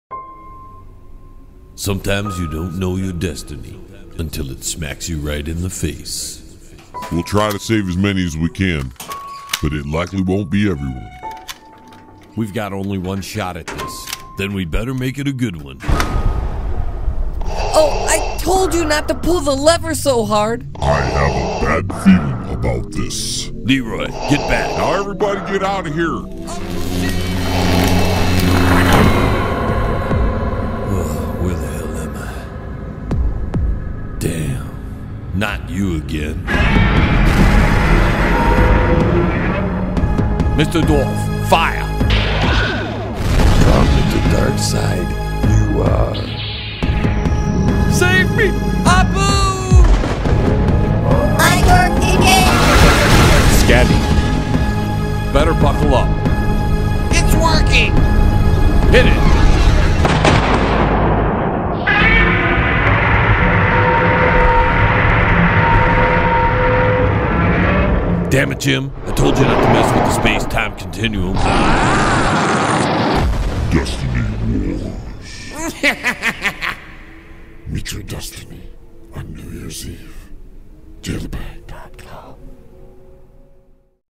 Destiny Wars (Radio Trailer)
The latest trailer for my upcoming radio drama. DESTINY WARS!!!

Our friends are back in their biggest adventure yet. With the balance of the universe on a razors edge... all they have left is their destiny.

Face Your Destiny on New Years eve.

Starring
John Lebang
Shady
Wilbert Hillbillie
Abu Mortoodi
Captain James Prichard
Doctor Herald McCoy
Mr. Tukok
Captain Rainbow
Darth Knight
Dirty Dean
The Bro's from da Hoe (Limp, Whimp, * Shrimp)
Nerdi Macgeek
Yogi
Leroy
Ditzy Stuper

Introducing
I.D.O.R.K.

Cameo's
Godzilla
Alex Jones
Juan Claude Van damme